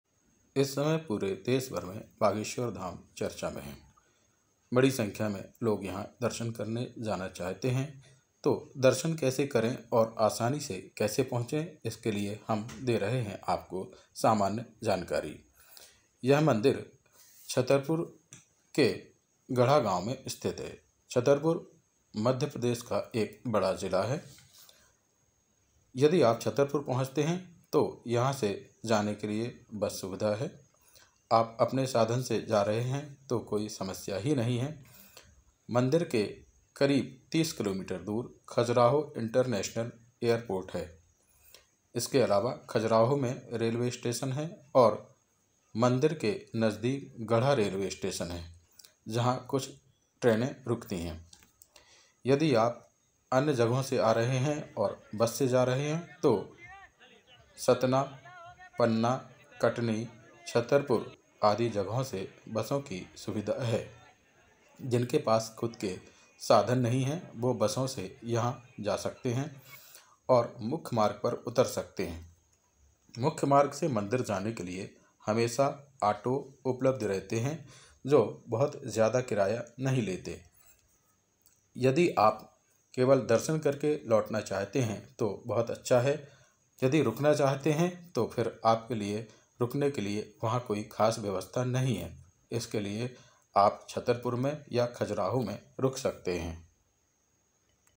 कैसे पहुंचे बागेश्वर धाम
बागेश्वर धाम इन दिनों देशभर में प्रसिद्ध है।पूरे देश से लोग यहां पहुंच रहे हैं।बागेश्वर धाम पहुंचने में लोगों को आसानी हो इसके लिए हम सामान्य संक्षिप्त जानकारी यहां दे रहे हैं।बागेश्वर धाम मध्य प्रदेश के छतरपुर जिले में स्थित है जो जिला मुख्यालय से करीब 35 दूर है और यहां पहुंचने के लिए पर्याप्त आवागमन के साधन उपलब्ध हैं।